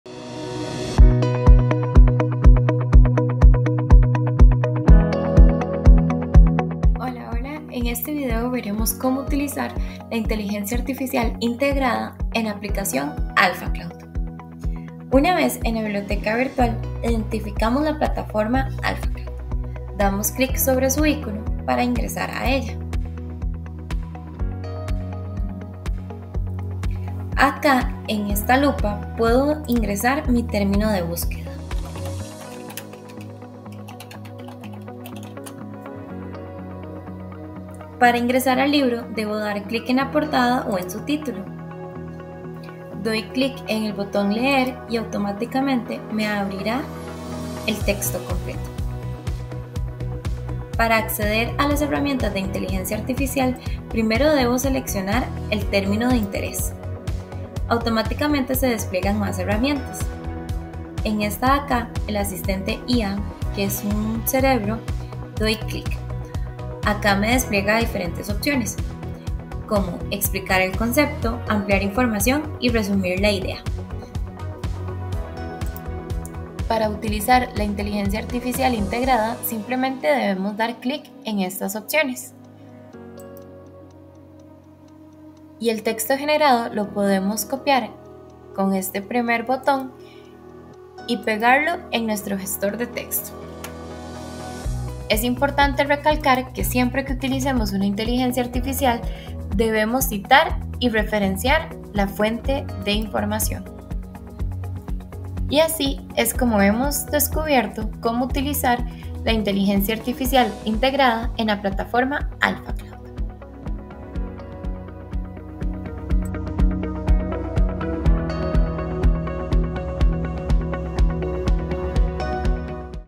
Uso de inteligencia artificial integrada en Alphacloud
Descubre como Ebooks 7-24 revoluciona las búsquedas de recursos en su plataforma por medio de la inteligencia artificial IAN.